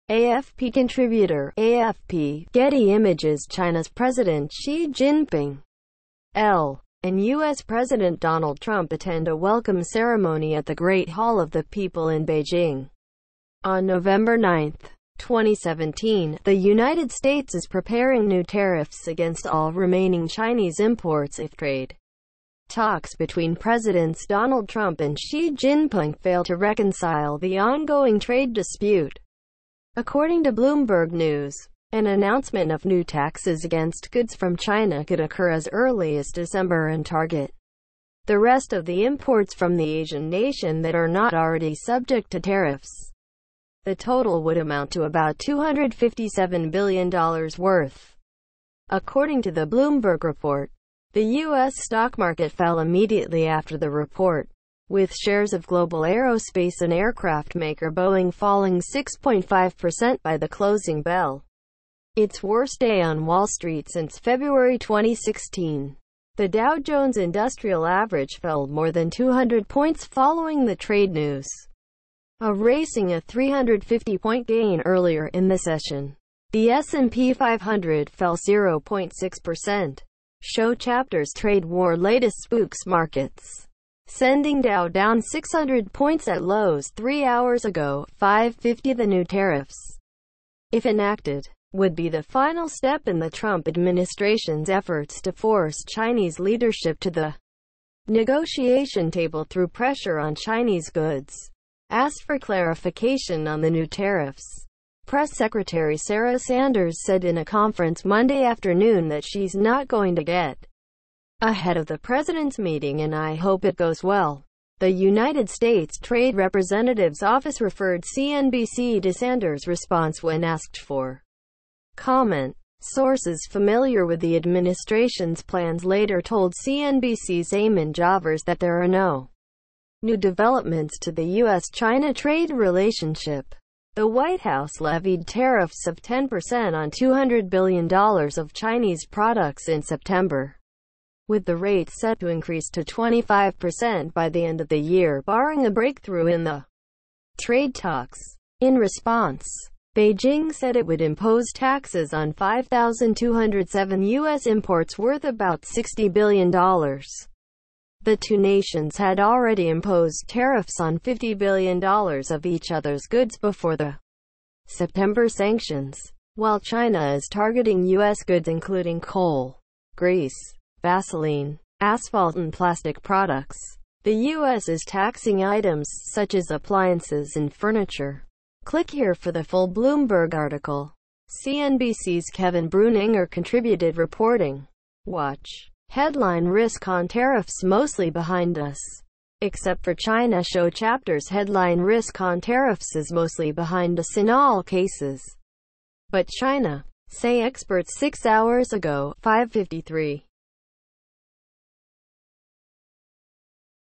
US reportedly planning tariffs on remaining $257 billion in Chinese goods if Trump-Xi talks fail
AFP Contributor | AFP | Getty Images Chinas President Xi Jinping (L) and US President Donald Trump attend a welcome ceremony at the Great Hall of the People in Beijing on November 9, 2017. The United States is preparing new tariffs against all remaining Chinese imports if trade talks between Presidents  Donald Trump  and  Xi Jinping  fail to reconcile the ongoing trade dispute, according to Bloomberg News. An announcement of new taxes against goods from China could occur as early as December and target the rest of the imports from the Asian nation that are not already subject to tariffs. The total would amount to about $257 billion worth, according to  the Bloomberg report. The U.S. stock market fell immediately after the report, with shares of global aerospace and aircraft maker  Boeing  falling 6.5 percent by the closing bell, its worst day on Wall Street since February 2016. The  Dow Jones Industrial Average  fell more than 200 points following the trade news, erasing a 350-point gain earlier in the session; the  S&P 500  fell 0.6 percent. show chapters                                       Trade war latest spooks markets, sending Dow down 600 points at lows                              3 Hours Ago |  05:50 The new tariffs, if enacted, would be the final step in the Trump administrations efforts to force Chinese leadership to the negotiation table through pressure on Chinese goods. Asked for clarification on the new tariffs, Press Secretary Sarah Sanders said in a conference Monday afternoon that shes not going to get ahead of the presidents meeting and I hope it goes well. The United States Trade Representatives office referred CNBC to Sanders response when asked for comment. Sources familiar with the administrations plans later told CNBCs  Eamon Javers  that there are no new developments to the U.S.-China trade relationship. The White House levied tariffs of  10 percent on $200 billion of Chinese products  in September, with the rate set to increase to 25 percent by the end of the year barring a breakthrough in the trade talks. In response, Beijing said it would impose taxes on 5,207 U.S. imports worth about $60 billion. The two nations had already imposed  tariffs on $50 billion of each others goods  before the September sanctions. While China is targeting U.S. goods including coal, grease, Vaseline, asphalt and plastic products, the U.S. is taxing items such as appliances and furniture. Click here for  the full Bloomberg article. CNBCs  Kevin Breuninger  contributed reporting. WATCH:  Headline risk on tariffs mostly behind us, except for China show chapters                                       Headline risk on tariffs is mostly behind us in all cases, but China, say experts                              6 Hours Ago |  05:53